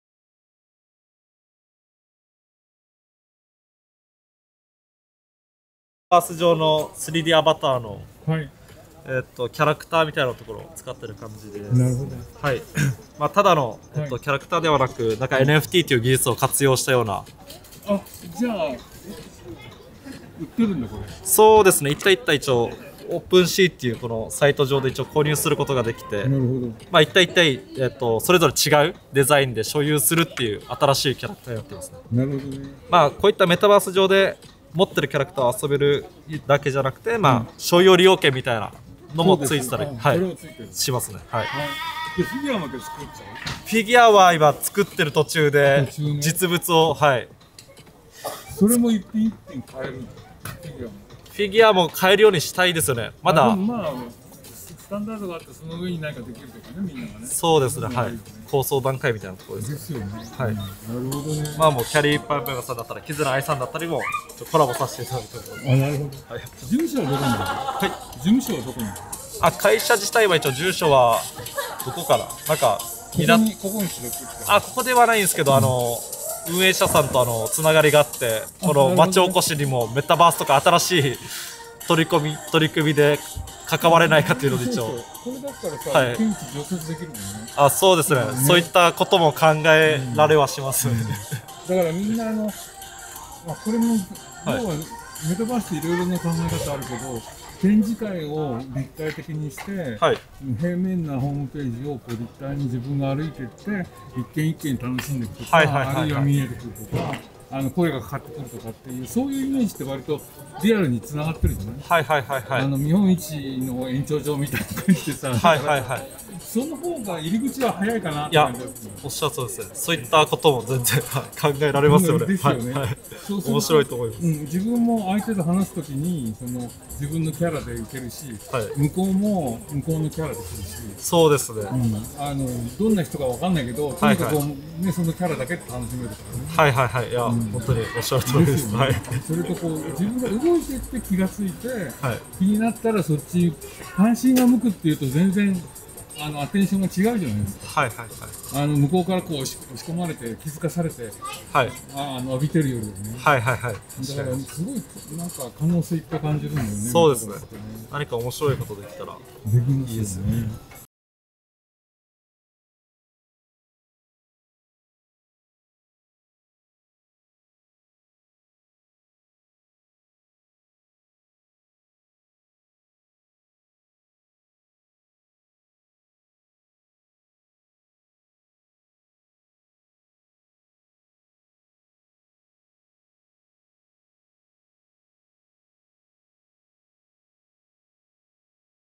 Metaani:3D NFTアバター 株式会社BeyondConcept at 日本橋問屋街 いっぴんいち
日本橋問屋街　いっぴんいち　にて
2023年1月22日(日)
主催：日本橋横山町・馬喰町参画推進プログラム、＋LOBBY日本橋問屋街
協賛：株式会社REGION、横山町馬喰町街づくり株式会社
協力：UR都市機構、問屋街有志、エンジョイワークス、パッチワークス
取材・記録：まちひとサイト